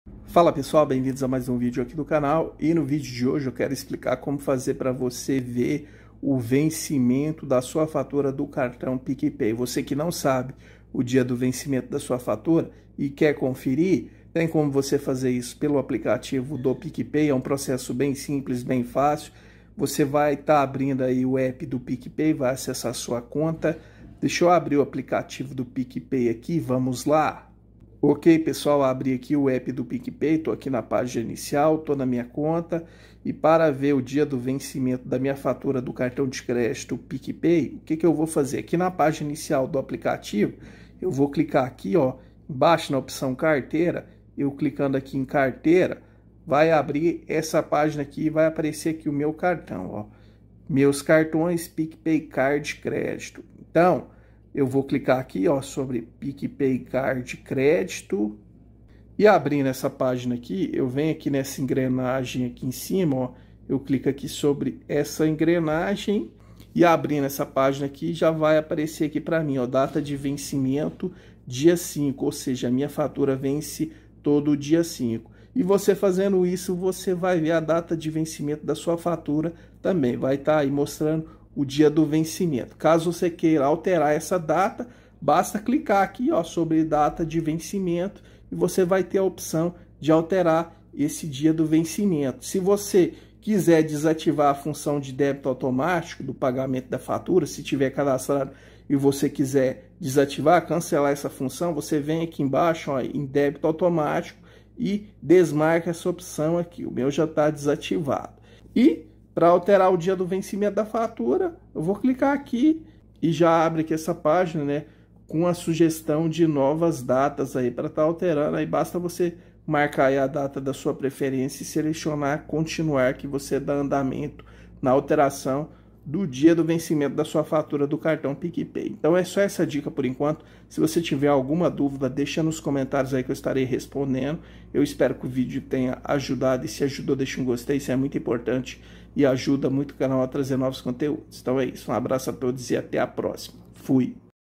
Como ver o vencimento da fatura PicPay
Neste vídeo eu explico como ver o dia do vencimento da fatura do Cartão de Crédito PicPay pelo aplicativo.